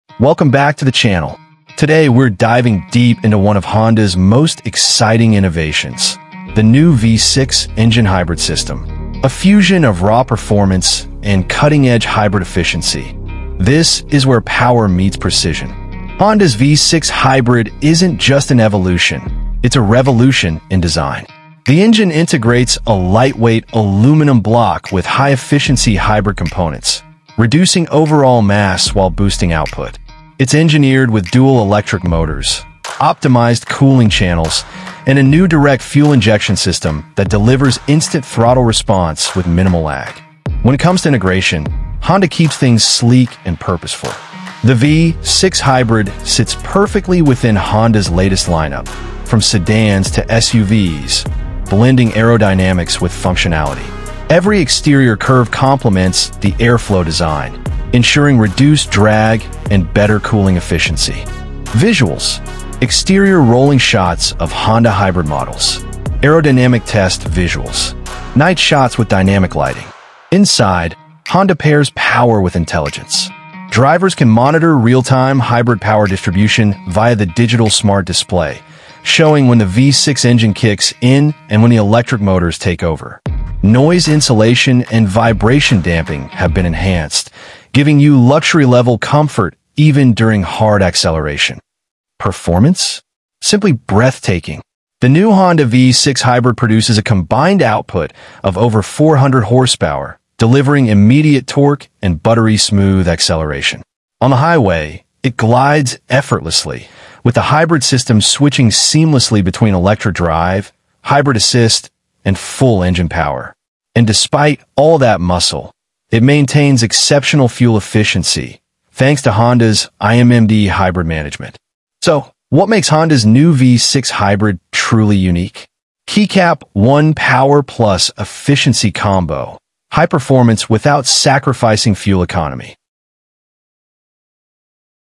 Honda New V-6 Engine Hybrid System | Next-Gen Power & Performance Innovation! 🚗⚡
Description:
Experience the future of performance with the Honda New V-6 Engine Hybrid System — where cutting-edge engineering meets hybrid efficiency. In this short 2-minute video, explore its bold design, futuristic interior, aerodynamic exterior, and unmatched highway performance. 🌿🔥
From its seamless hybrid powertrain to the thrilling V6 roar, Honda redefines the modern driving experience!

Timestamps:
00:00 – Intro: Honda’s Vision for the Future 🚀
00:15 – Design & Exterior: Bold and Aerodynamic ✨
00:45 – Interior: Premium Comfort & Smart Tech 🧠
01:15 – Performance: V6 Hybrid Power Unleashed ⚡
01:45 – Unique Selling Points & Final Thoughts 💎
02:00 – Outro: Drive the Future with Honda 🌍

Why Watch This:
👉 Discover Honda’s latest V6 hybrid innovation.
👉 See the perfect balance between power and sustainability.
👉 Get a close-up of the stunning design and interior luxury.
👉 Learn what makes this engine system a game-changer for the automotive world.